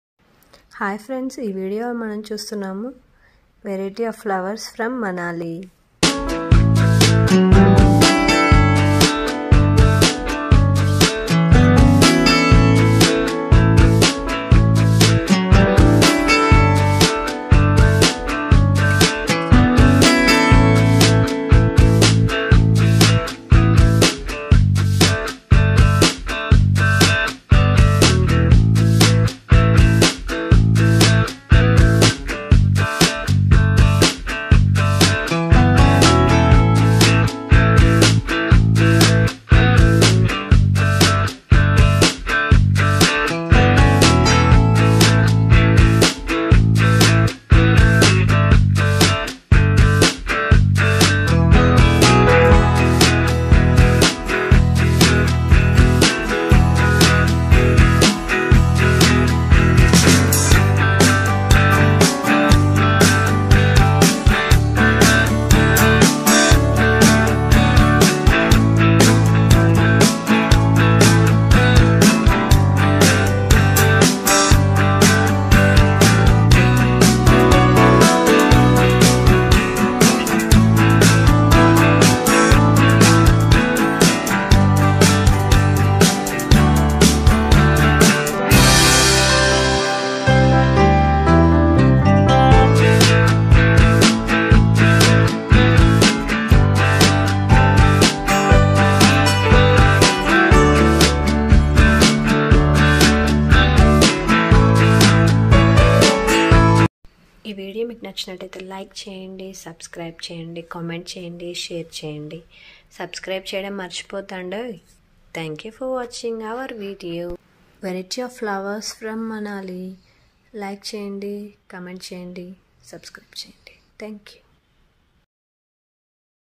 variety (రకరకాల పువ్వులు)of flowers from manali l beautiful flower garden#manali
Manali tour from delhi,Delhi to Manali roadways, variety of flowers from manali himachal pradesh.
valley of flowers from manali
valley of flowers